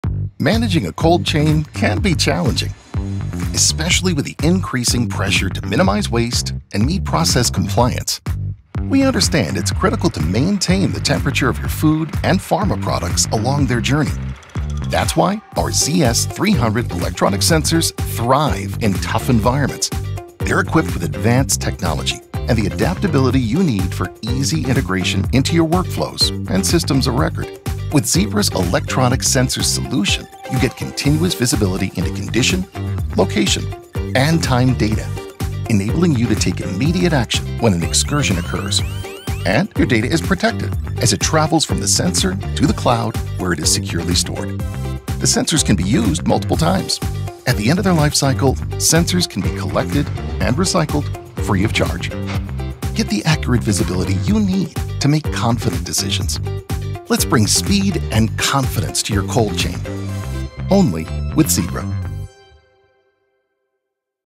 ZS300 Electronic Sensors | Zebra
Introducing the ZS300 Sensor and ZB200 Bridge, an electronic temperature solution. In this video, we delve into the challenges of managing a cold chain, particularly in minimizing waste and meeting compliance. The ZS300 electronic sensors are well suited for harsh environments. These sensors are equipped with advanced technology and adaptability for seamless integration into your workflows and systems. Zebra's electronic sensor solution offers continuous visibility into condition, location, and time data, allowing you to have the data you need to make informed decisions. Your data is securely transferred to the cloud and stored safely. These environmental sensors can be used multiple times and are recyclable at the end of their lifecycle, free of charge. Gain the accurate visibility you need to make confident decisions in your cold chain management with Zebra's ZS300 Sensor and ZB200 Bridge. Interested in learning about temperature indicators? To find out what temperature indicators are, check out